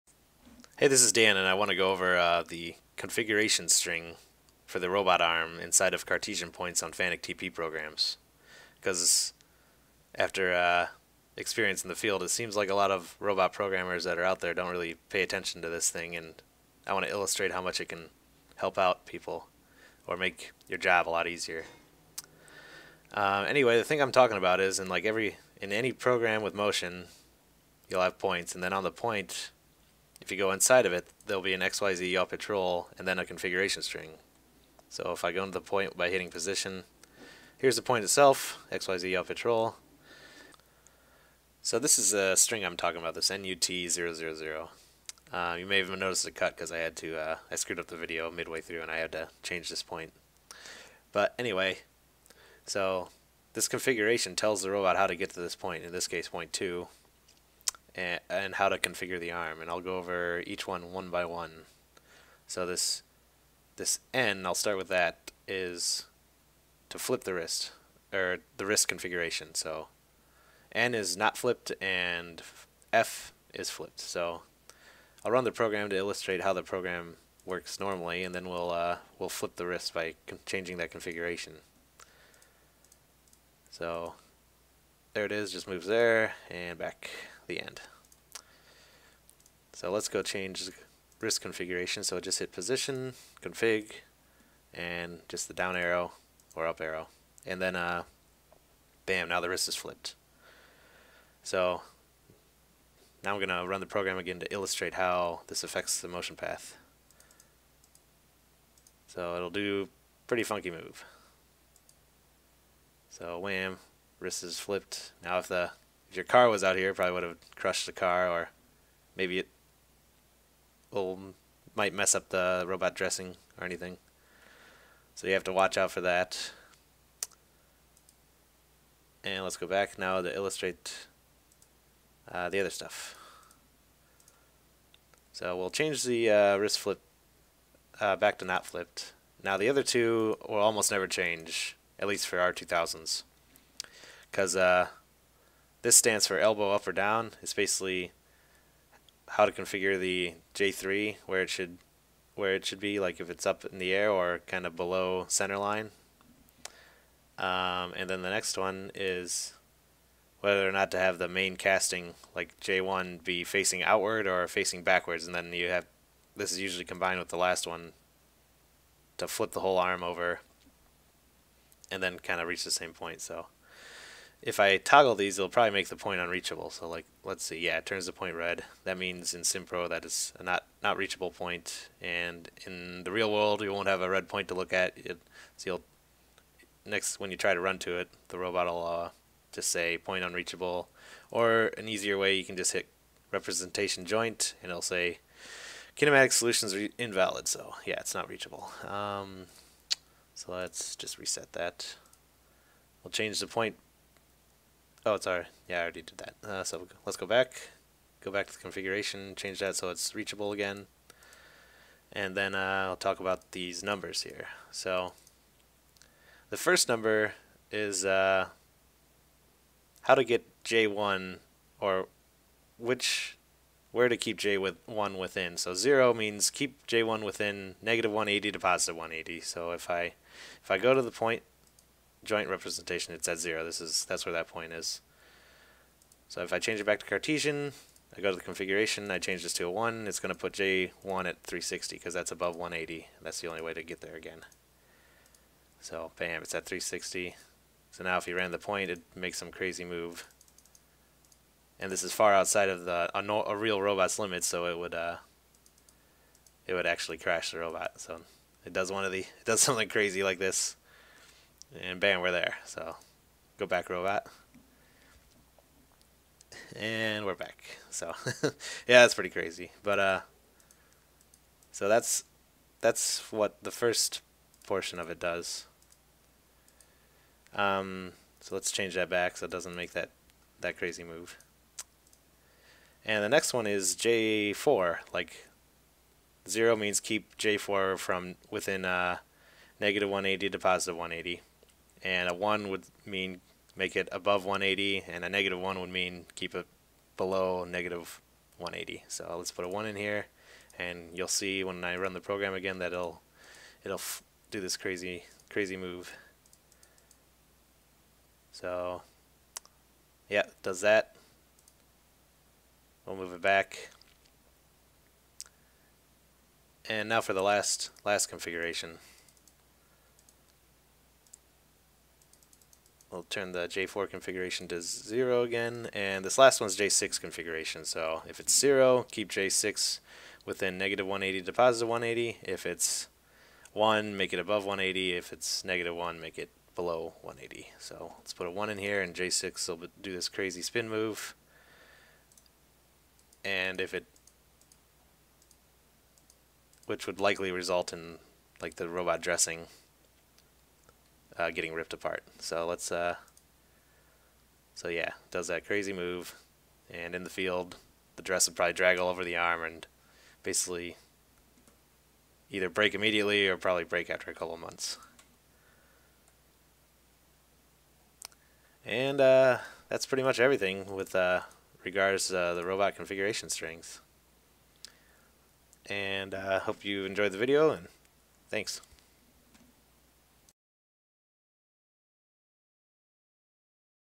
Fanuc Robots: The Configuration String
In this video I go over what the configuration string inside of Cartesian points on Fanuc TP programs does, and how to manipulate it.